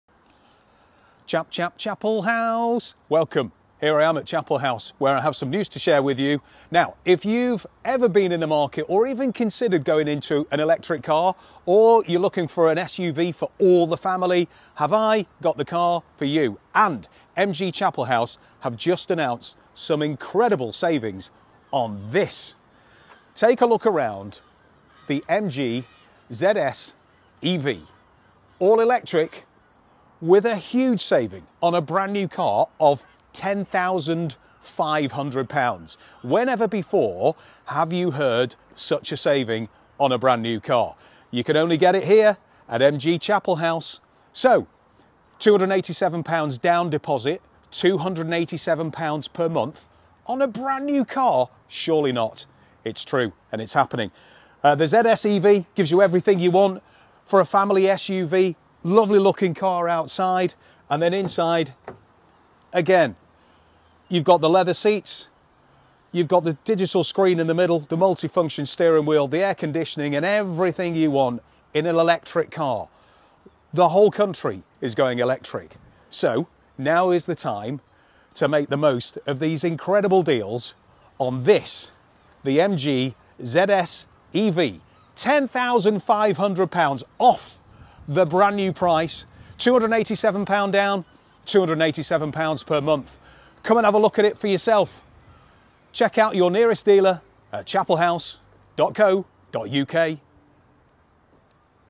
SAVE £10,500 ON A BRAND NEW MG ZS EV! 😮 Here's Darren Proctor to show you around!
Find out more about our range of new and used cars

To find out more about our stores take a look at our Brands and dealerships: